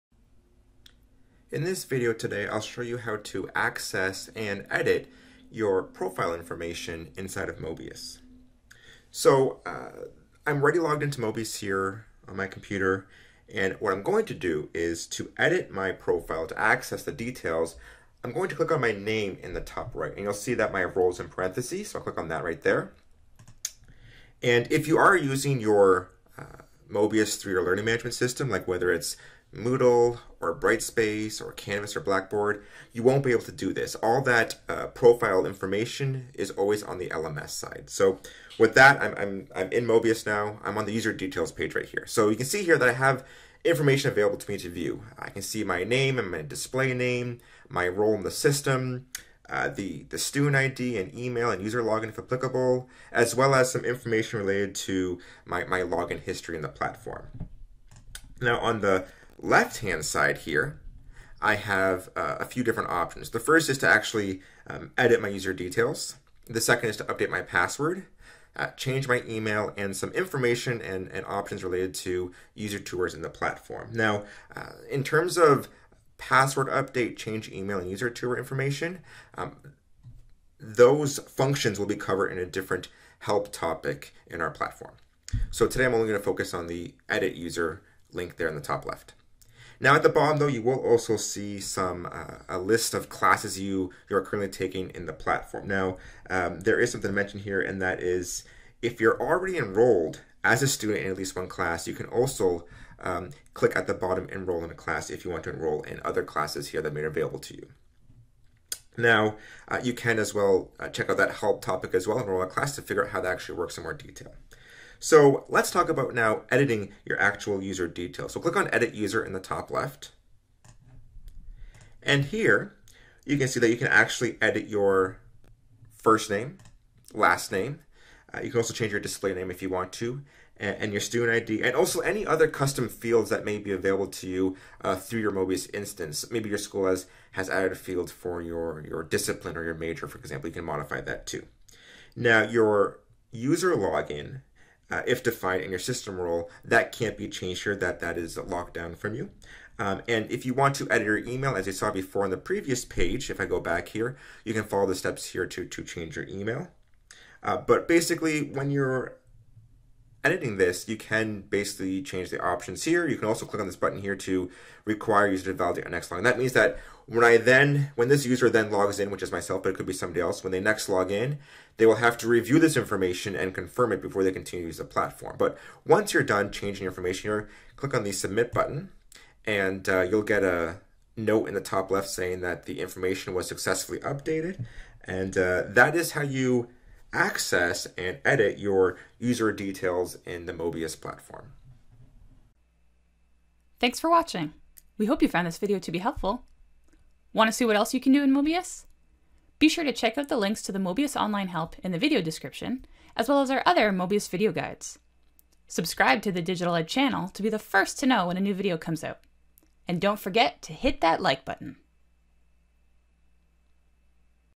Manage my user profile | Möbius Video Guides by DigitalEd
Learn how to access and edit your Möbius user profile with this step-by-step video!

Want more details?
where you can read the “Access and edit my user profile” help topic